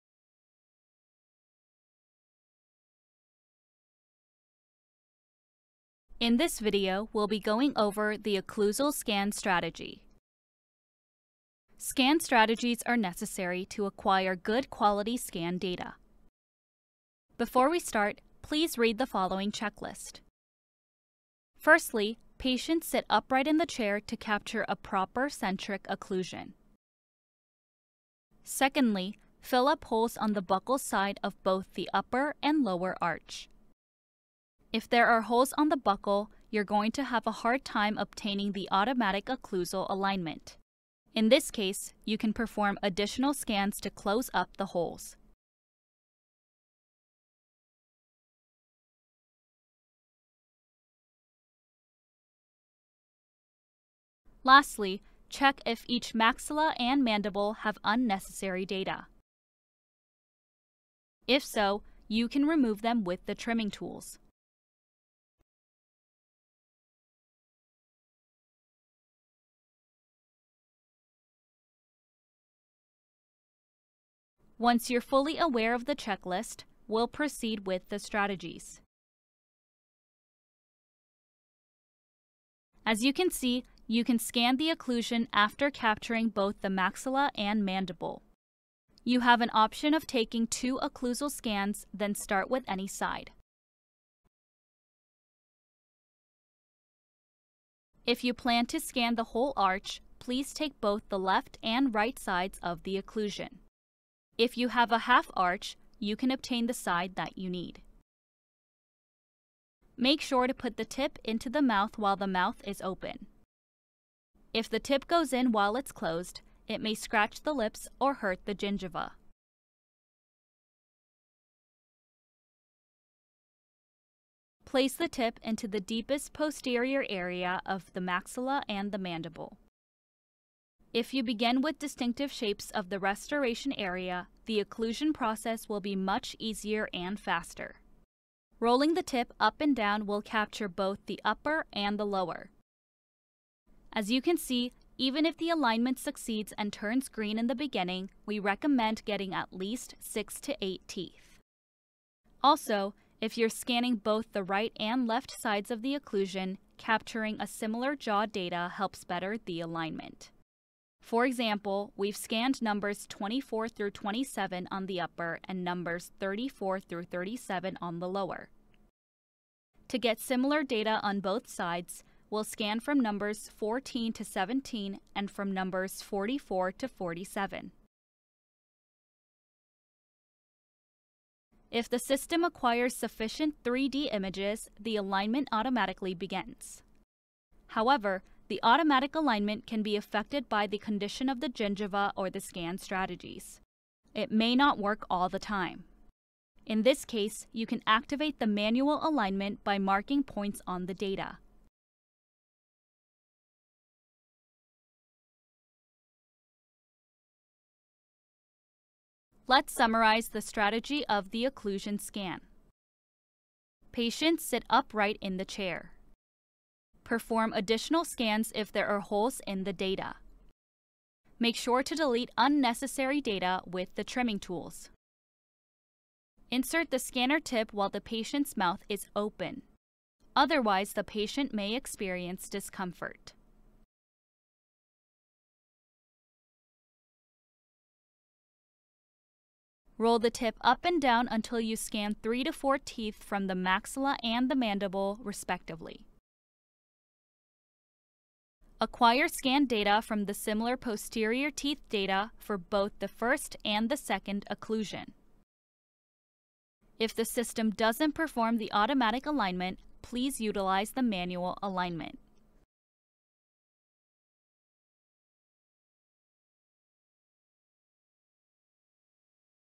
Scan Strategy_Occlusion (ENG)
- This video explains scanning strategies and useful tips when scanning occlusions.